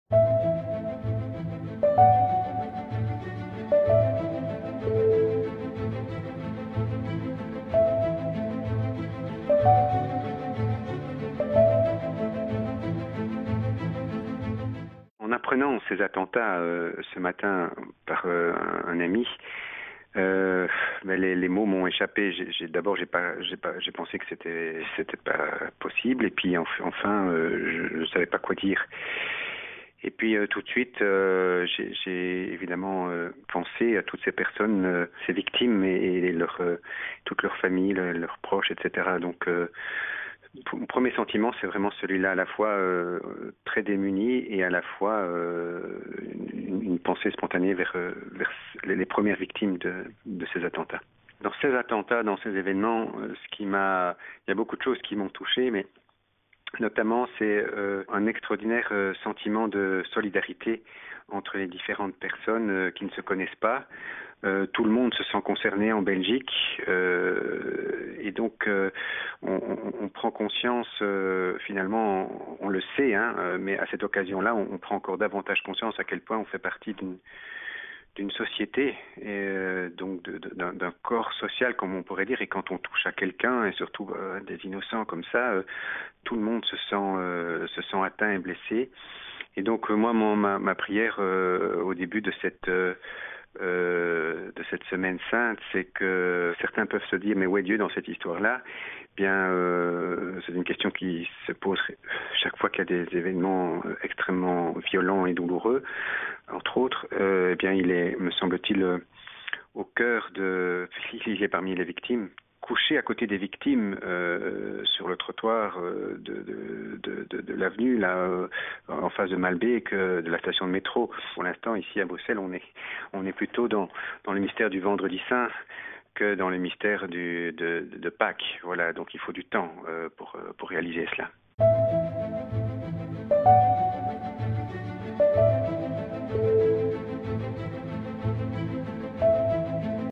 PrayForBrussels : Père Olivier Bonnewijn
"Tout le monde se sent atteint et blessé", affirme le père Olivier Bonnewijn, membre de la communauté de l'Emmanuel et professeur à l'Institut d'études théologiques de Bruxelles. Un drame qui plonge "dans le mystère du vendredi saint", ajoute le prêtre.
Flash du 22/03/2016.